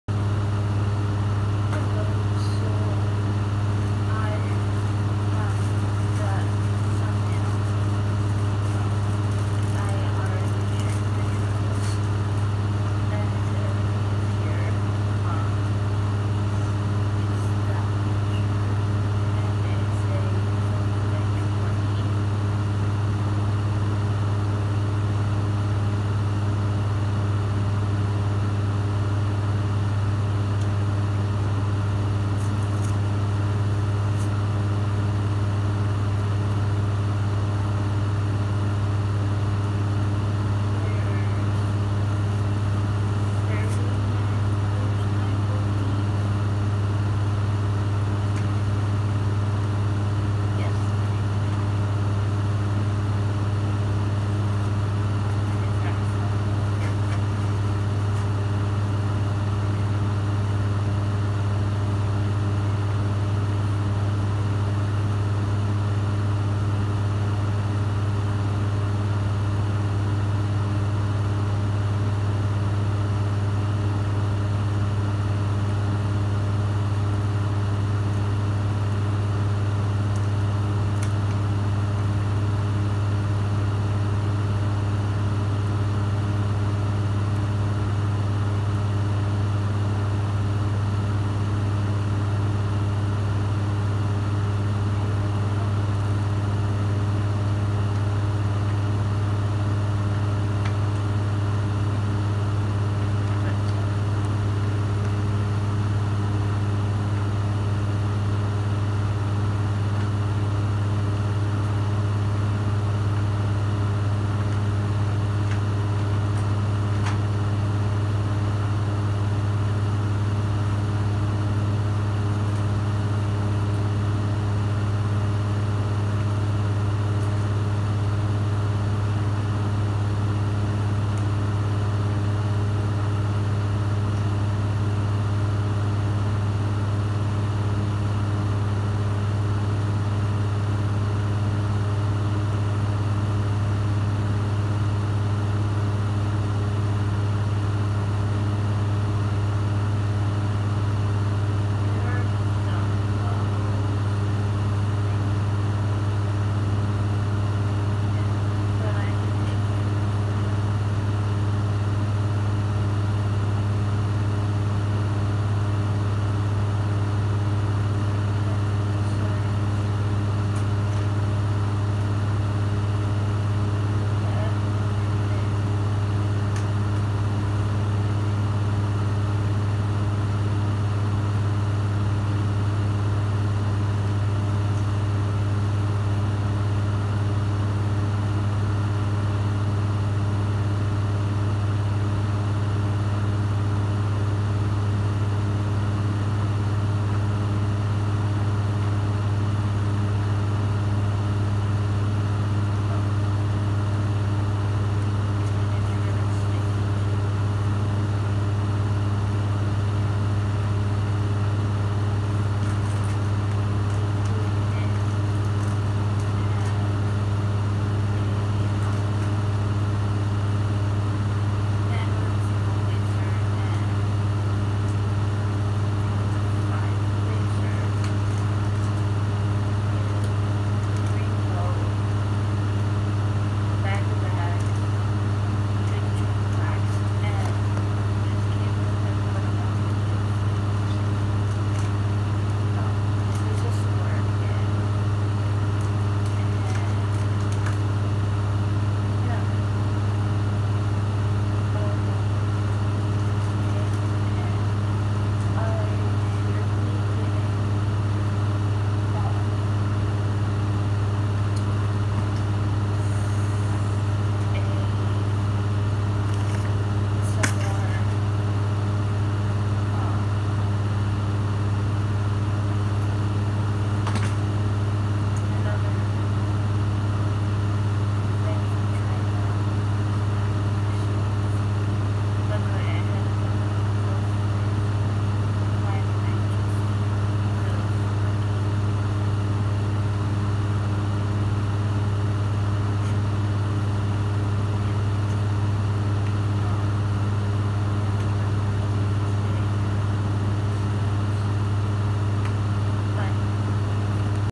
Un bagging a care bear diamond painting
Got this yesterday. I had the fan on so sorry if you can’t hear me. I was having a hot flash.